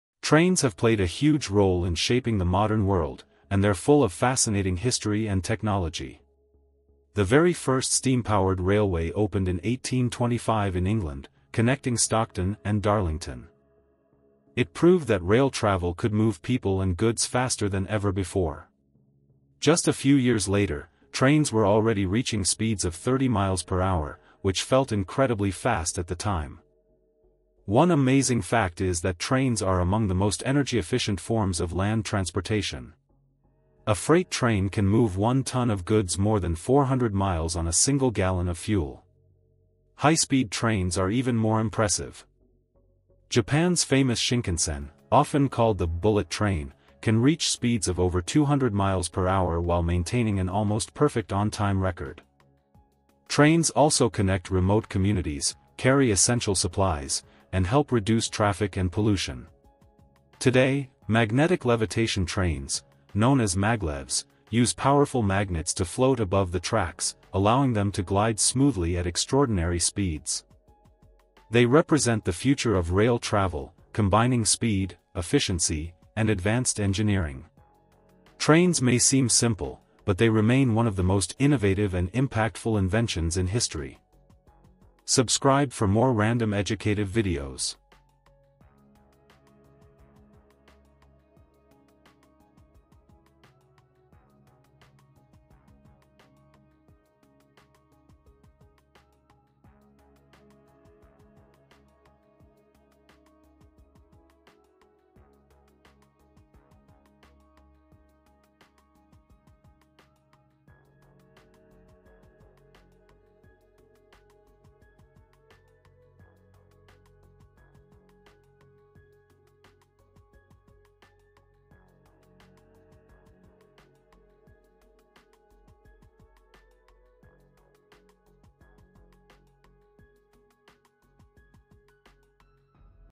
Did You Know: Trains are Among the most Energy-Efficient Forms of land Transportation 🚂
Thank you guys so much for watching your  @DailyShortOfEducationalFacts    ,
The best spot for random educative facts. Hope you enjoy !

That really helps the channel reaching more curious minds 🔭

Have a nice day! See you tomorrow 🙂